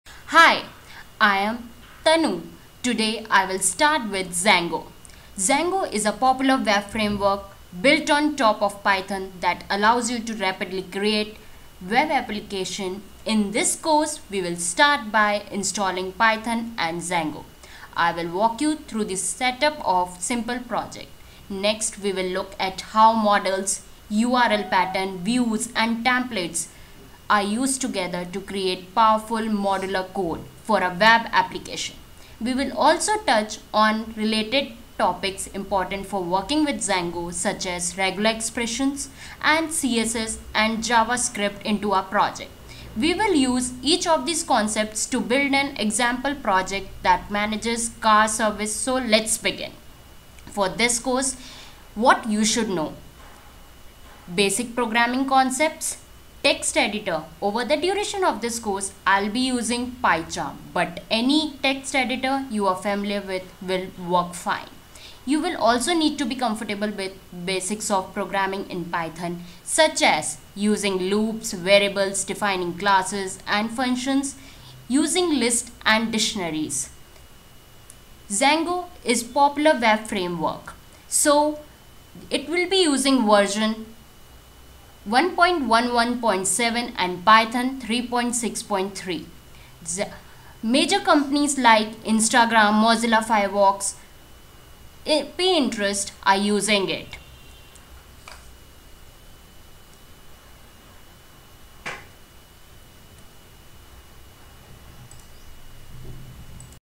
Django Basics: The Ultimate Beginner's Guide
With Django, you can take web applications from concept to launch in a matter of hours. Django is a free and open-source framework that is designed on top of Python and supports data-driven architecture. Learn what you need to know to get up and running with Django with these tutorials from instructor Tanu. Tanu walks through creating a brand-new Django project, defining a data model and fields, querying the database, and using Django's built-in URL handlers, views, and templates to structure the rest of the backend. Plus, learn how to incorporate CSS and JavaScript to enhance the style and usability of your Django templates.

If anything is unclear, please let me know in a comment.
Happy Learning!!!

0:00: Introduction
0:06: An outline of this video
0:52: Prerequisite for Django
1:18: Version of Python and Django we will be using?
1:30: Major Companies who are using Django Framework

                1(a) What You Should know?
                1(b) What is Django?
Chapter 2: SETTING UP A DJANGO PROJECT
                2(a) Download Python and Django
                2(b) Create a Django Project
                2(c) Create a Django App
Chapter 3: WORKING WITH DJANGO MODELS AND ADMIN
                3(a) Models, Routing, Views and Templates
                3(b) Django Models and Fields
             .  3(c) Implementing Models and Fields
                3(d) Django Migrations
                3(e) Load Data in Sqlite
                3(f) Configure Admin
                3(g) Query Data with Django ORM
Chapter 4: URL HANDLERS AND VIEWS
                4(a) Understand URL Patterns
                4(b) Implement URL Patterns
                4(c) Implement Django Views
Chapter 5: DJANGO TEMPLATES
                5(a) Overview of Django Templates
                5(b) Implement Django Templates
Chapter 6: DESIGNING PROJECT
                6(a) Bootstrap Overview & Installation
                6(b) Implement Layout Bootstrap
                6(c) Implement Static Files
                6(d) Collect Static File
                6(e) Add CSS,JS,Jquery to static
                6(f) Finishing Touch to Template